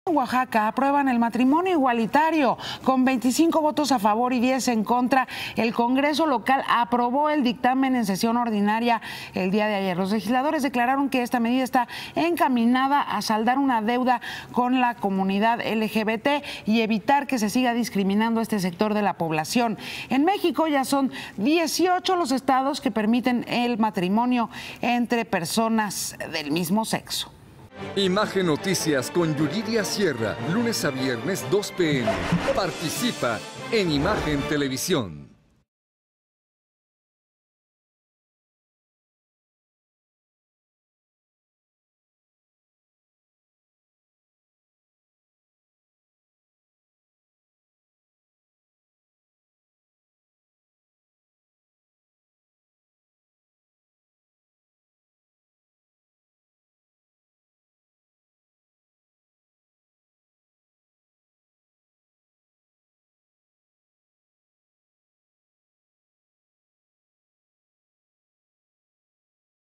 Aprueban el matrimonio igualitario en Oaxaca | Noticas con Yuriria Sierra
Congreso del Estado de Oaxaca aprobó el matrimonio igualitario en la entidad, con 25 votos a favor y 10 en contra.